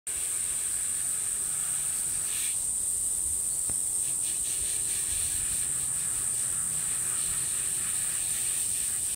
Irish Wolfhound X Bull Arab cooling off
Taking an outdoor shower in the heat of summer